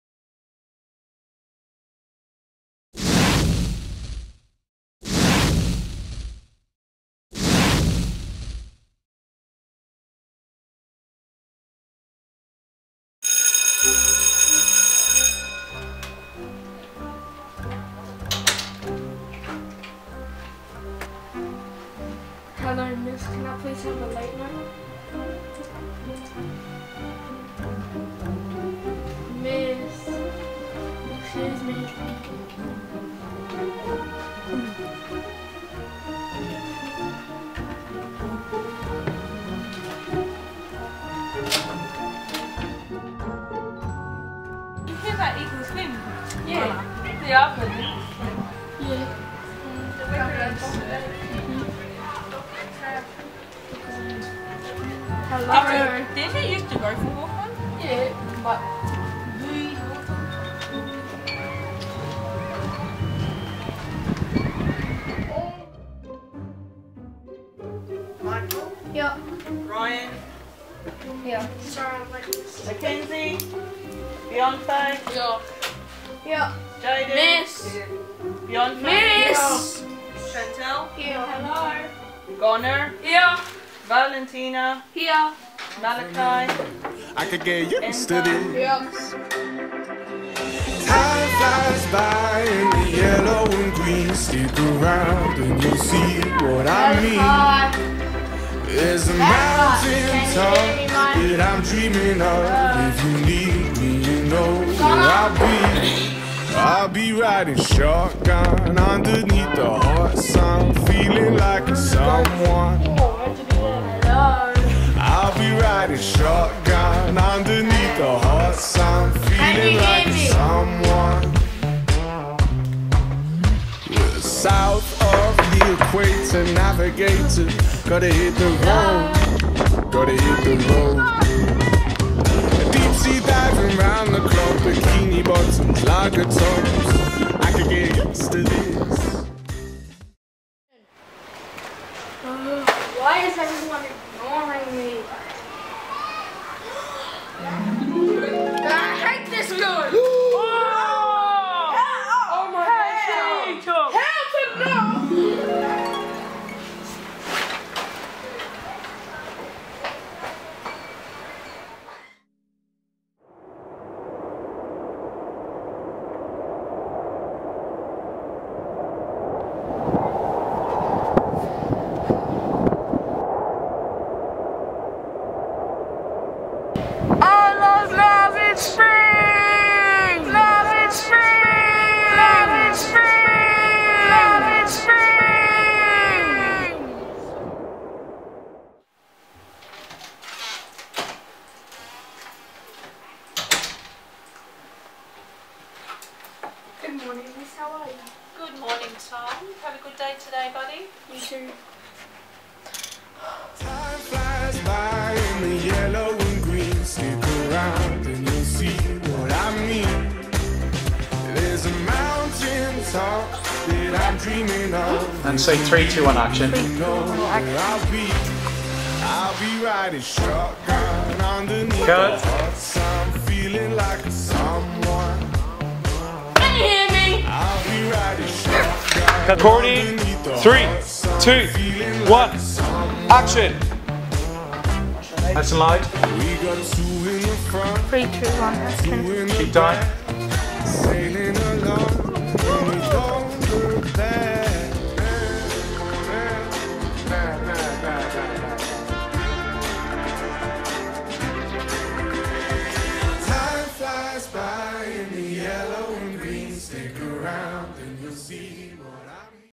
Look At Me! - A Student Short Film
A Short Film created by Year 6 boys on the Excellence Programme at Malvern Springs Primary School.  The film is about a boy becomes invisible around the School.